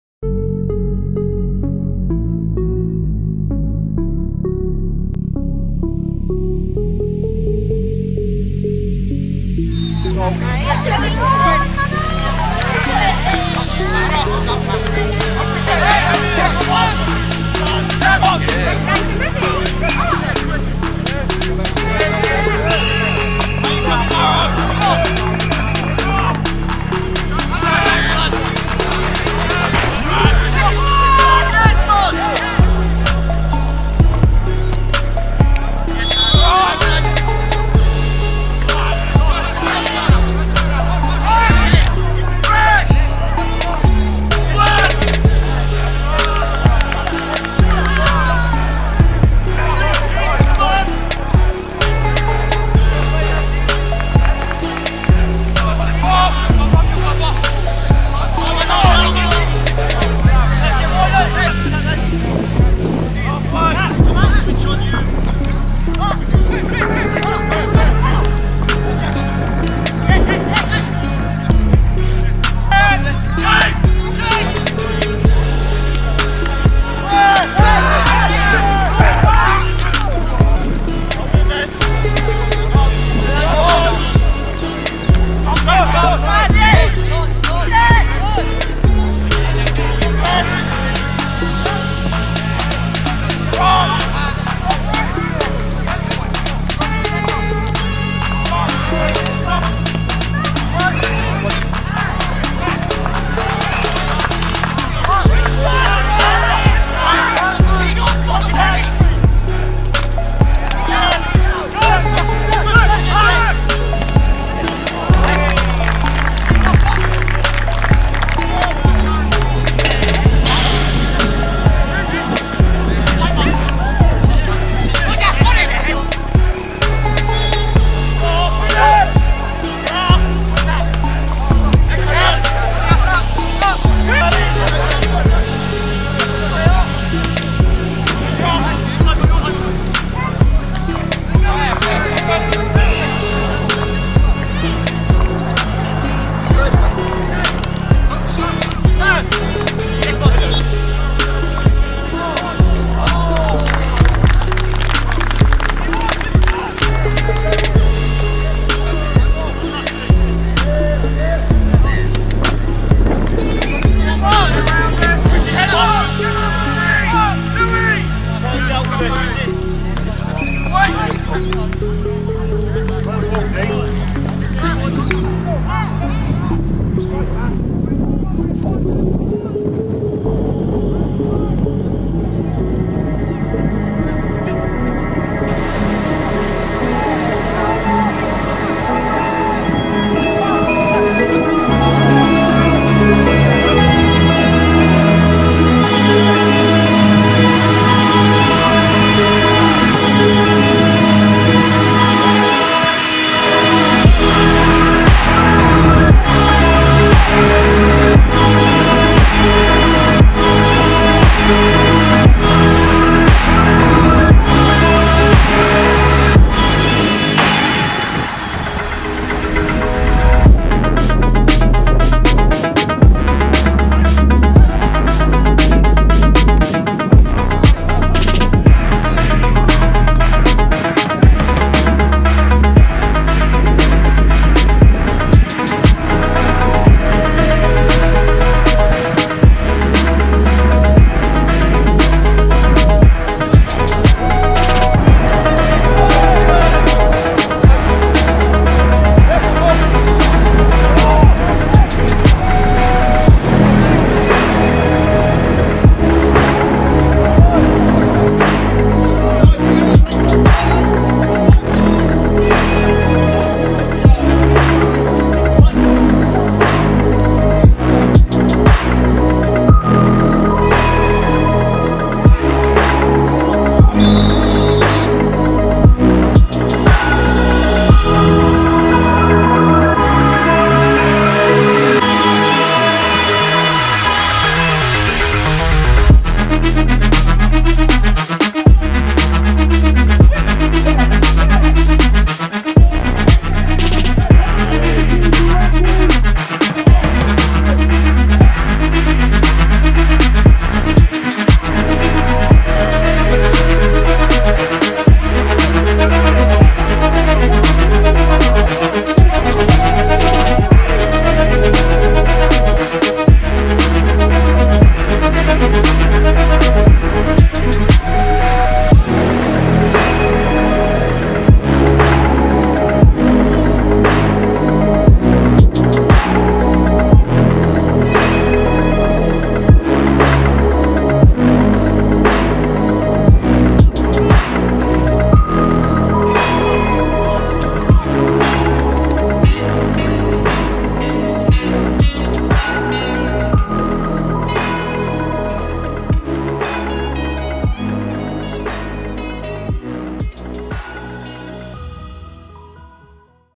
Patrick Millard Highlights 2019
Clips from Stevenage Football Club Youth Academy
Patrick Millard 17 years old
(6'3", 88kg)